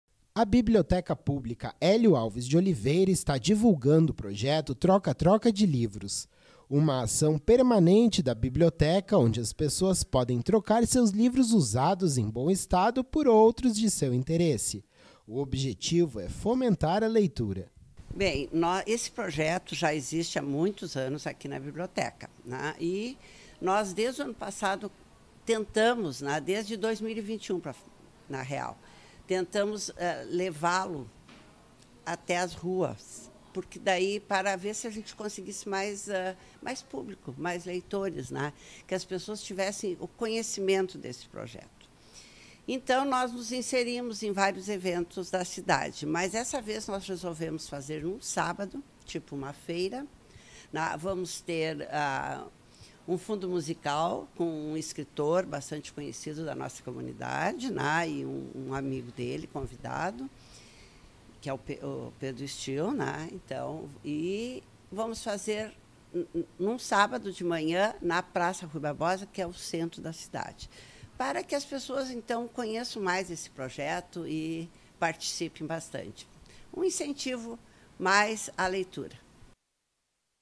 Projeto troca-troca de livros da Biblioteca
A Biblioteca está divulgando o projeto Troca-troca de Livros. Se você tem em casa algum livro usado que esteja parado na estante é hora de trocar esta obra por outra. Saiba como você pode participar desta ação.